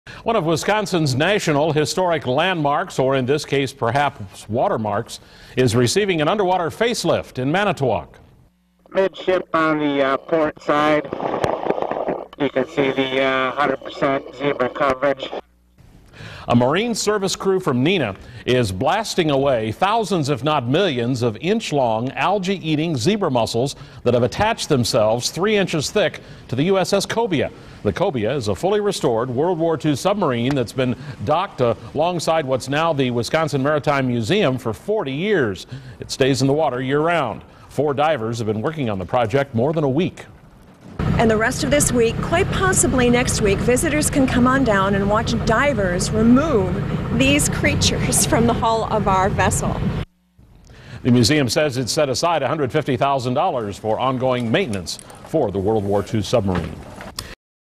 Zebra Mussel Blasting
Divers blasting off zebra mussels off the USS Cobia